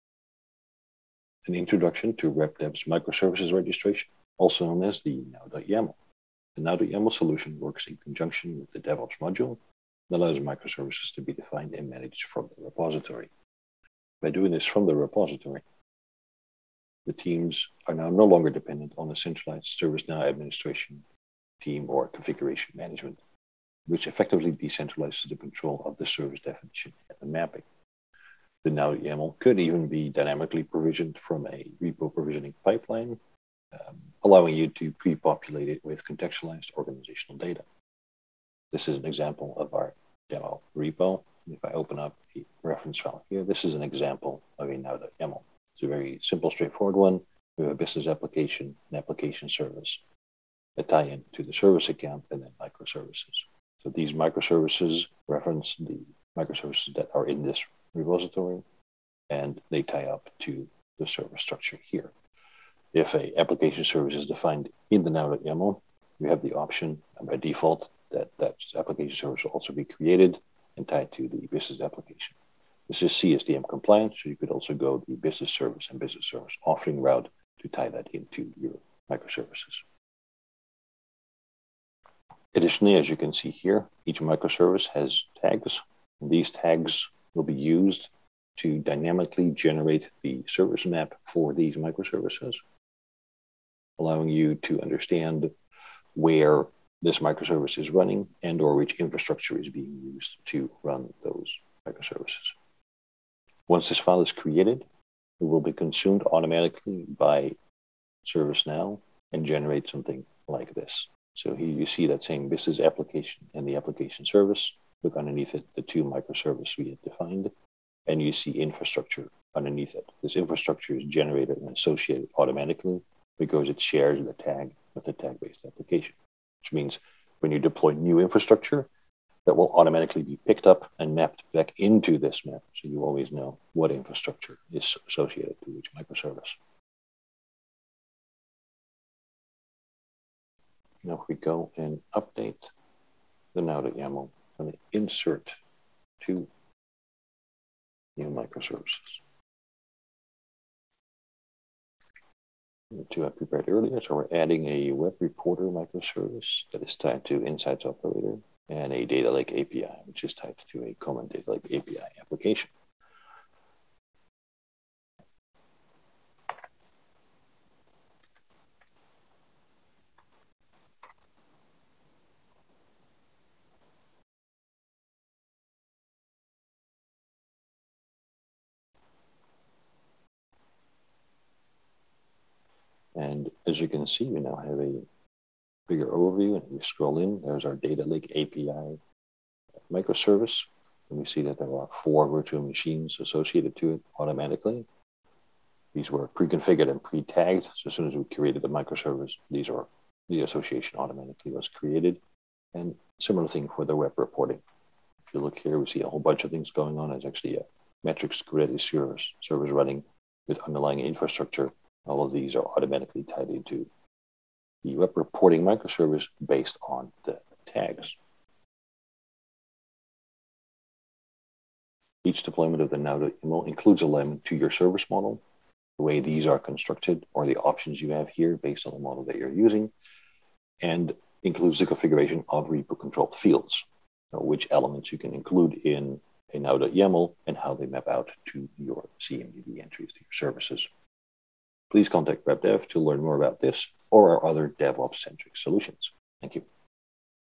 Microservices Registration Automation | RapDev ServiceNow Demo
Watch a quick walk-through of our now.yaml solution features that work in conjunction with ServiceNow's DevOps module.